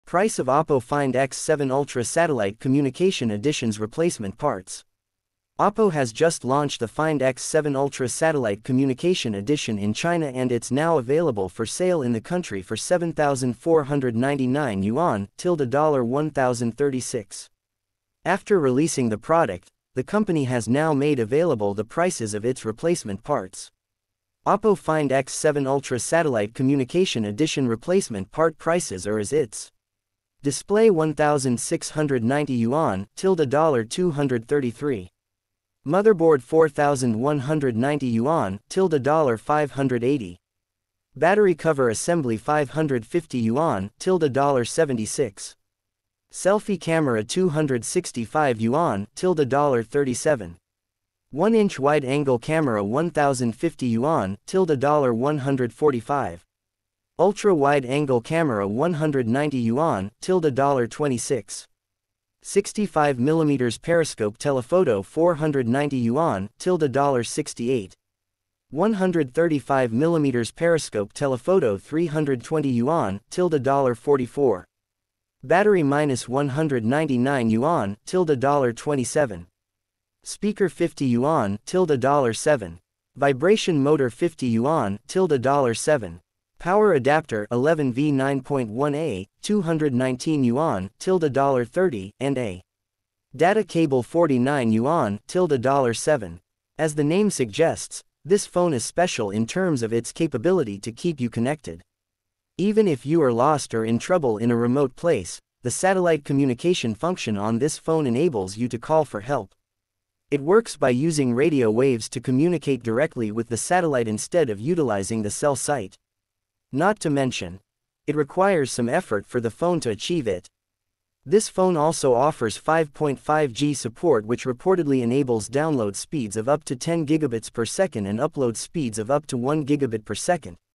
Price of Oppo Find X7 Ultra Satellite Communication Edition’s replacement parts.
Oppo has just launched the Find X7 Ultra Satellite Communication Edition in China and it’s now available for sale in the country for 7,499 yuan (~$1036). After releasing the product, the company has now made available the prices of its replacement parts.